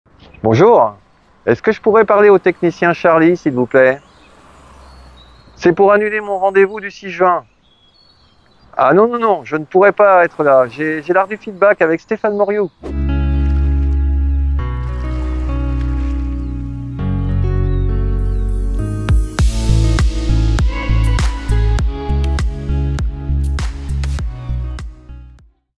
Les 20 ans de DUOTECH : "Je peux pas, j'ai feedback !"
DUOTECH INFORMATIQUE BRESSUIRE organise pour ses 20 ans des évènements à destination de ses clients

Une semaine de folie du 14 au 17 mai 2024 avec des conférences sur la digitalisation, les RH, la cyber, et l'intelligence artificielle.

Le 6 juin 2024 venez vivre une expérience différente avec le conférencier Stéphane MORIOU sur "L'art du Feedback", plus d'informations